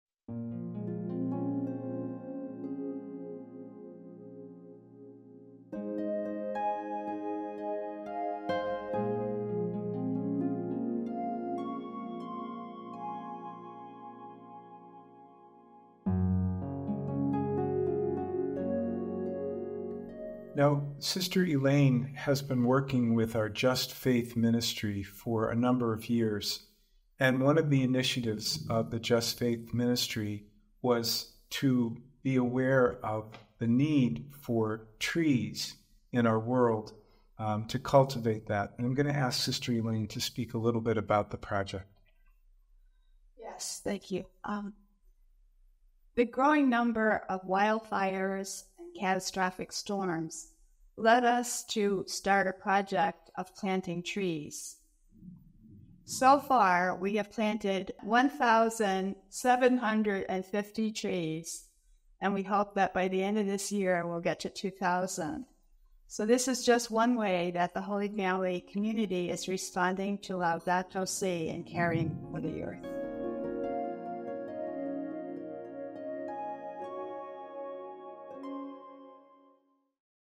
Living Laudato Si' at Holy Family Passionist Retreat Center in West Hartford, CT.
Each Friday during this Season of Creation, we will hear how Holy Family Retreat Center in West Hartford, CT is living Laudato Si’. This week, hear how planting trees has brought hope to all of the Passionist Family.